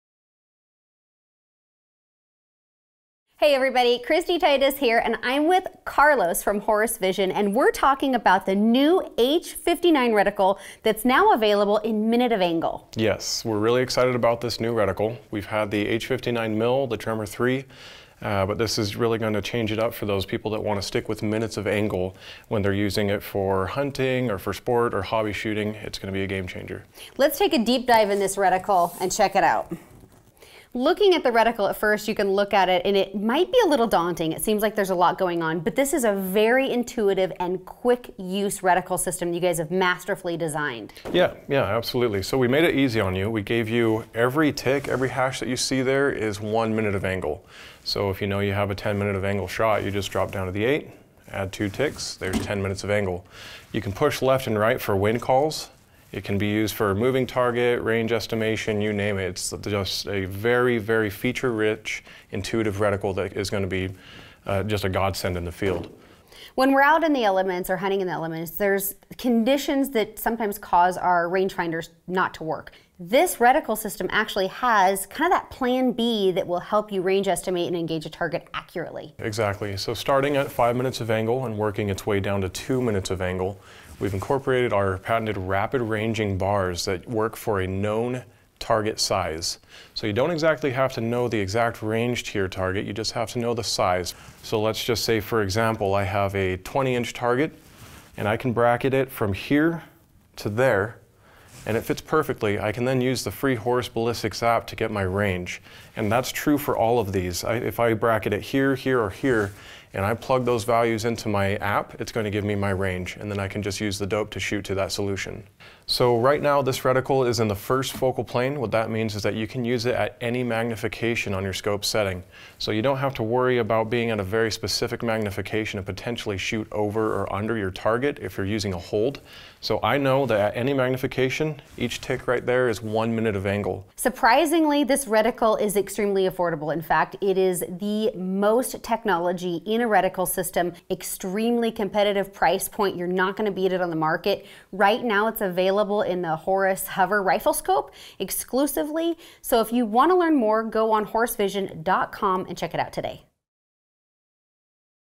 Horus Vision H59-MOA Reticle with Kristy Titus
Looking to try a HORUS reticle? In this episode of Pursue The Wild, Tips From The Wild, join Kristy Titus and Carlos Raff with Horus Vision to learn about the revolutionary H59™ reticle NOW available in MOA!

With a 1 MOA Horus Grid and the patented Rapid Range bars it provides incredibly fast and easy wind and drop compensation, target range estimation, and second shot correction capabilities making it ideal for those looking for an advanced reticle system calibrated in MOA.

The NEW H59™ reticle in MOA is available exclusively in the Horus HoVR 5-20x50 First Focal Plane is the scope.